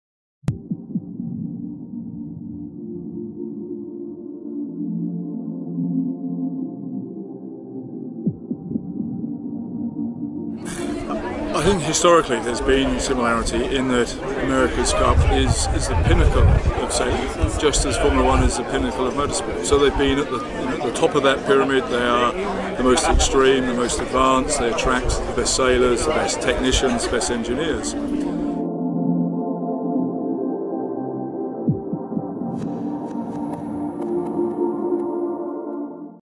Martin Whitmarsh dans Followed.
Le patron du défi anglais pour la coupe de l'america nous explique tout.